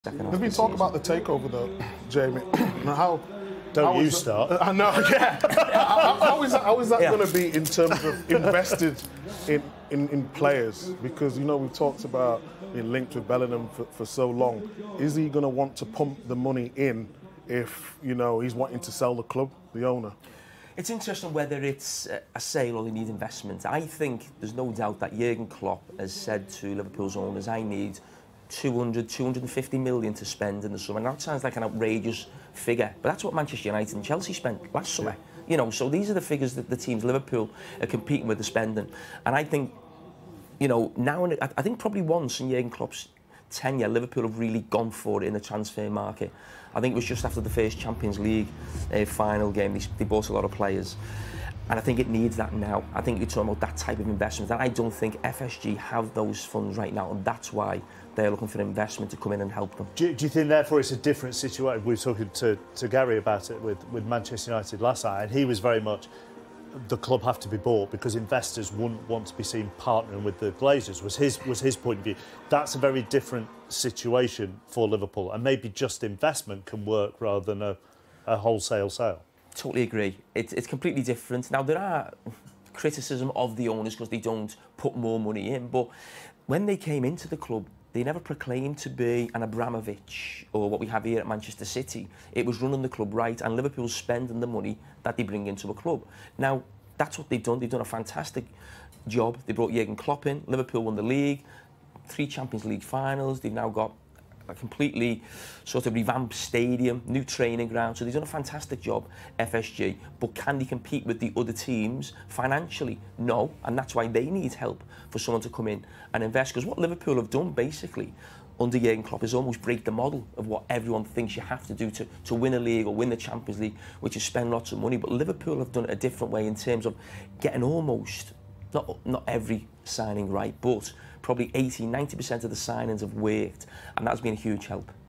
'Klopp will want £250 million' 👀 | Carra on Liverpool's ownership going forwards
Jamie Carragher says Liverpool's owners FSG have done a 'fantastic' job guiding the club over the last decade but admits they might seek help for future investments.